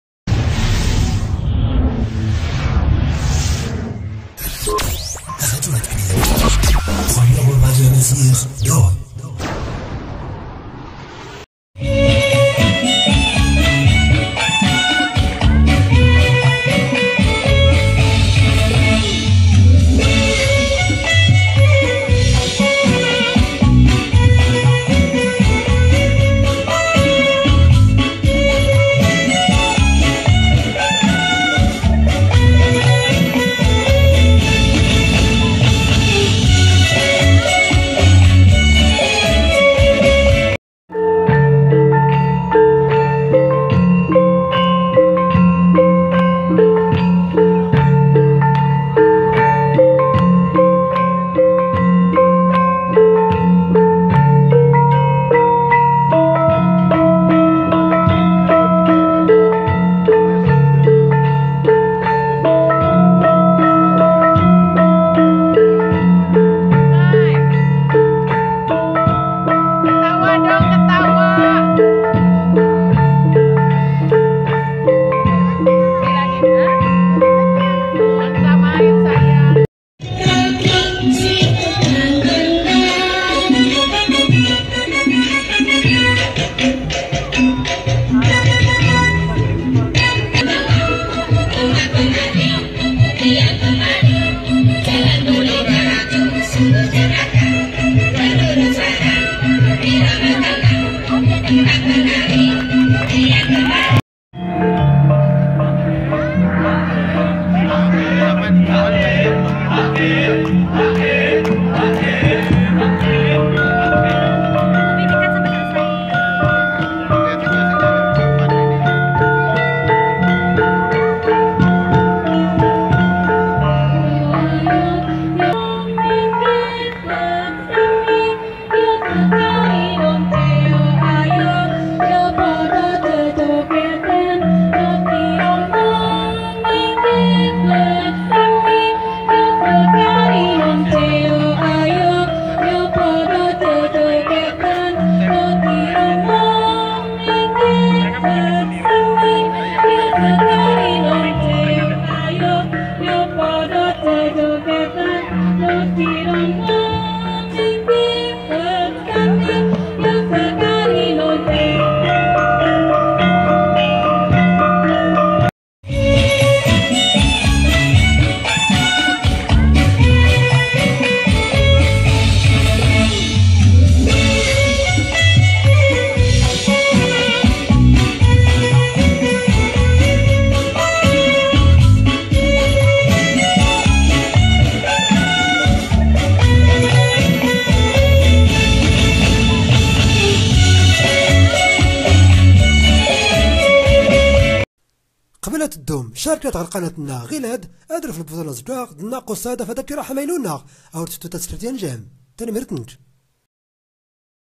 رقصات من الثراث الاندونيسي في إطار مهرجان المعكرونة فى دورته التانية بالرباط
راديو اتبير الأقرب إليكم